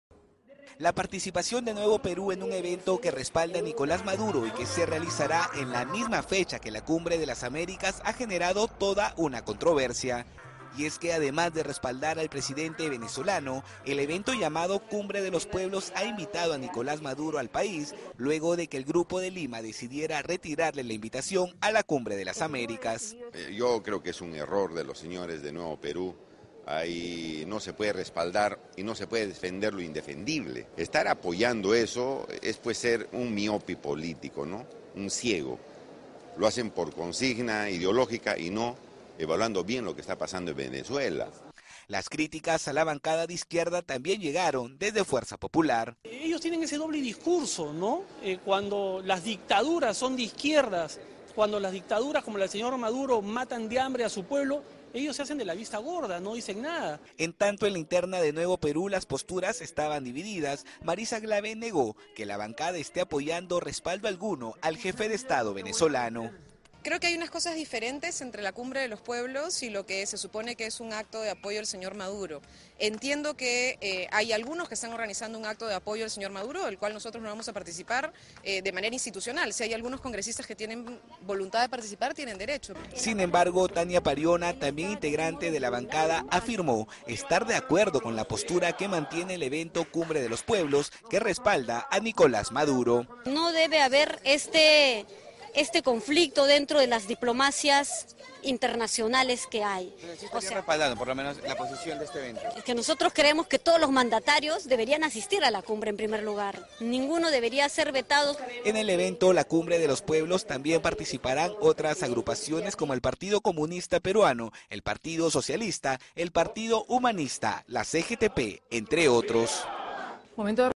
Invitación a Nicolás Maduro a “Cumbre de los Pueblos” en Lima genera controversia
La participación de Nuevo Perú en un evento que respalda a Nicolás Maduro y que se realizará en la misma fecha de la Cumbre de las Américas ha generado toda una controversia, ya que el evento llamado “Cumbre de los Pueblos” ha invitado a Maduro al Perú luego que el Grupo de Lima decidiera retirarle la invitación a la Cumbre de las Américas.

Las críticas a la bancada de izquierda no se hicieron esperar, por su parte el congresista Yonhy Lescano señaló que “apoyar eso es ser un miope político […] lo hacen por una consiga ideológica y no evaluando bien lo que esta pasando en Venezuela”; el parlamentario Daniel Salaverry también se refirió al respecto y dijo que “cuando las dictaduras son de izquierda […] ellos (integrantes de Nuevo Perú) se hacen de la vista gorda”.

Mientras tanto, internamente en Nuevo Perú las posturas están dividas, por un lado la legisladora Marisa Glave negó que su bancada este apoyando respaldo alguno al jefe de estado venezolano. “Creo que hay unas cosas diferentes entre la Cumbre de los Pueblos y lo que se supone es un acto de apoyo al señor Maduro”.

Sin embargo, Tania Pariona, también integrante de Nuevo Perú afirmó estar de acuerdo con la postura que mantiene el evento “Cumbre de los Pueblos” que respalda al mandatario venezolano.